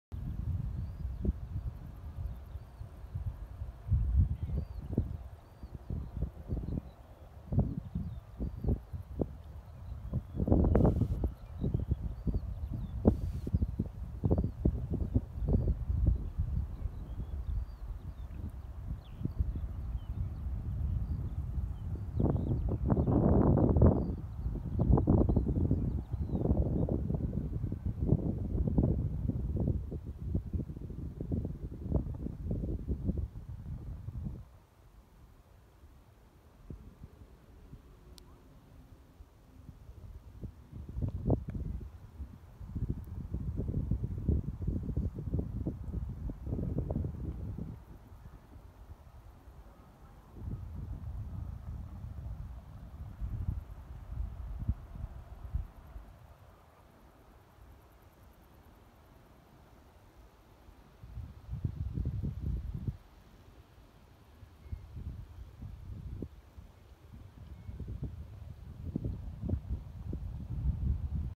Soundscapes of Stockholm (6)
In Stockholm there is a large field where people come and relax, and do sports. It it also the host of many different events, such as an annual kite flying event. There is also pastures with horses here, even though it is just five to ten minutes with a buss from the central station in Stockholm. Up until 1994 the field was also home to the Royal Sheep who walked here. It is a bit of a rural oasis in the middle of Stockholm.

Photo and recording by Erik Pålsson.